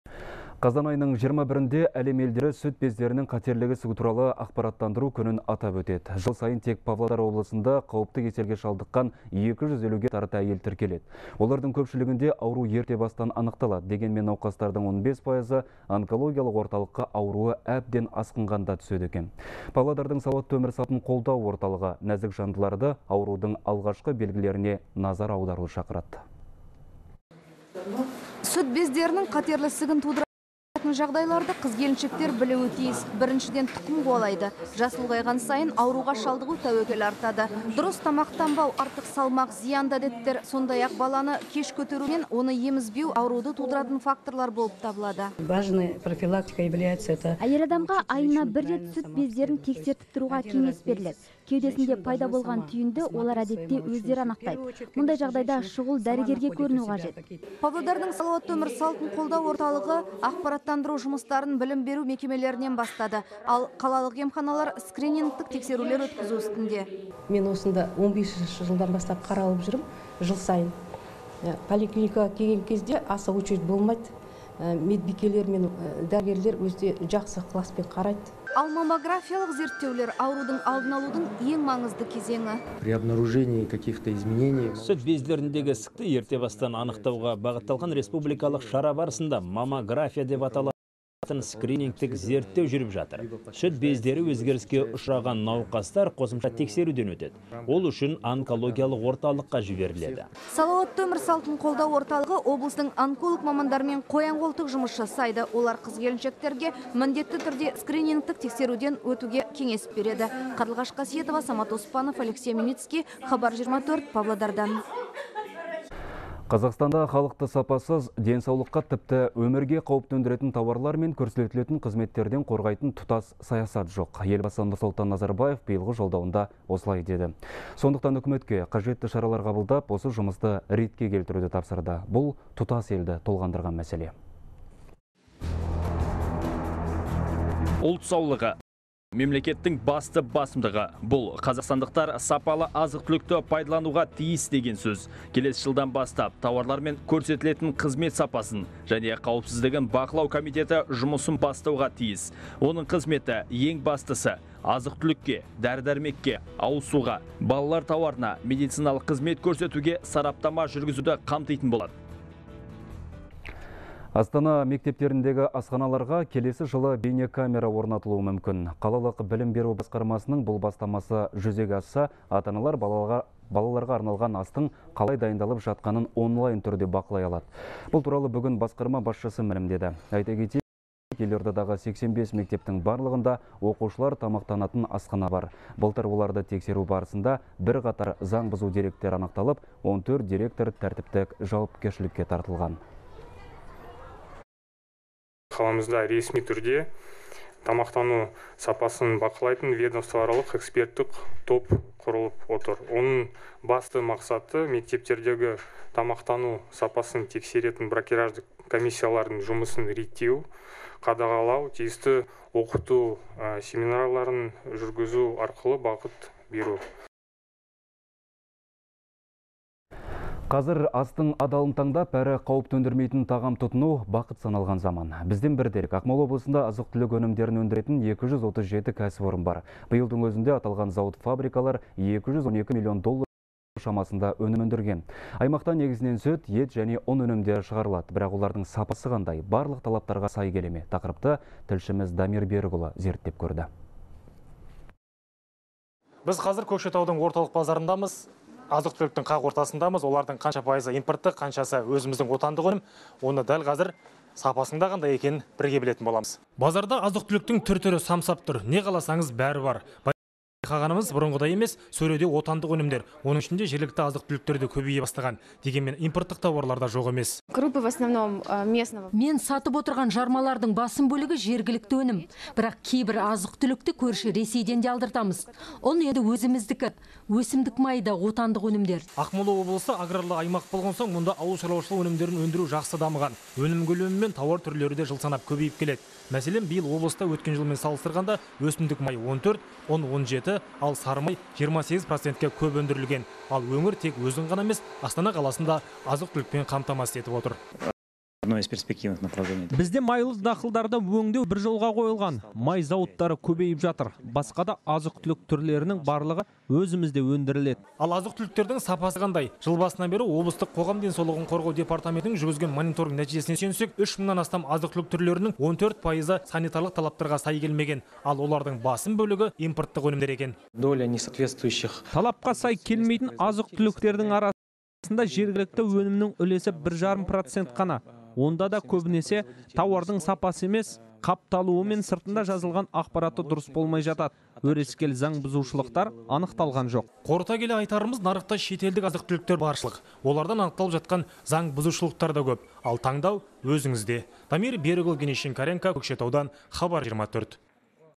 Павлодарда сүт бездері қатерлі ісігінің алдын алу шаралары қаралды
Қазан айының 21 жұлдызында әлем елдері сүт бездерінің қатерлі ісігі туралы ақпараттандыру күнін атап өтеді.